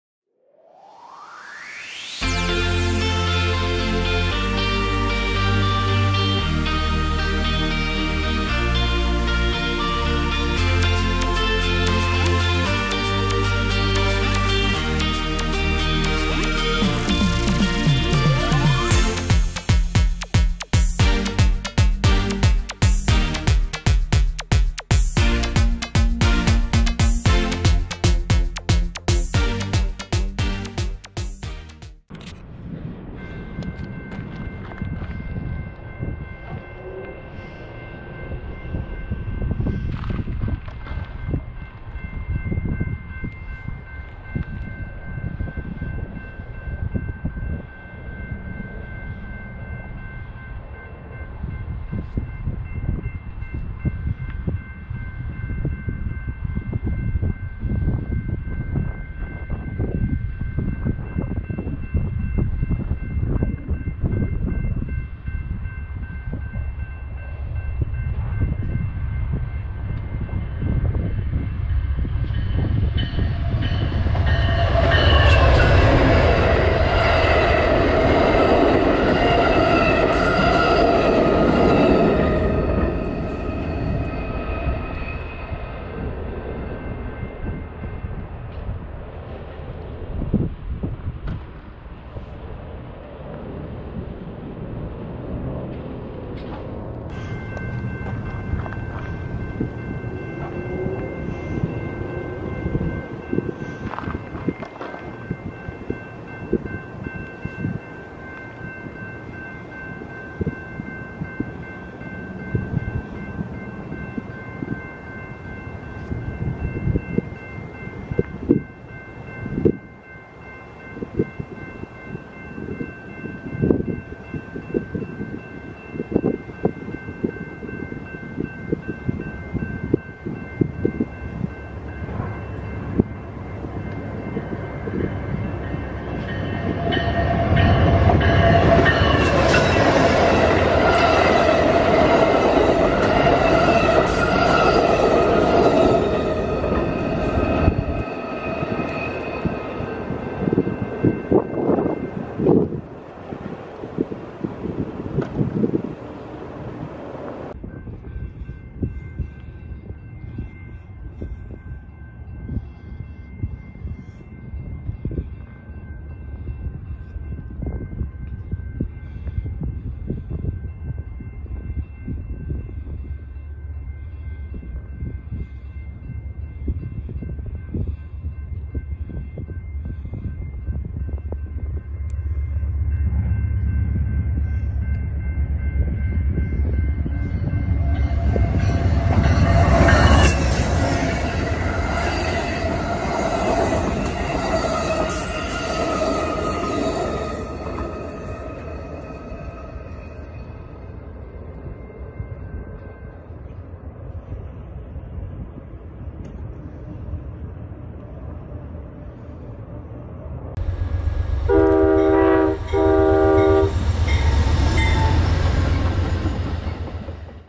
Amtrak 8 into Wenatchee, through the Smoke and Darkness
At the end of our Labor Day weekend adventures, we couldn't head home due to wildfires and were stuck in Wenatchee. I pulled out the camera gear and caught Amtrak 8, the Empire Builder, arriving into Wenatchee amongst the smoke filled skies and the evening darkness.  AMTK 161 led the builder into Wenatchee this evening.

MP 1651.93 - Miller St Crossing
BNSF Columbia River Subdivision
Wenatchee, WA
9/7/2020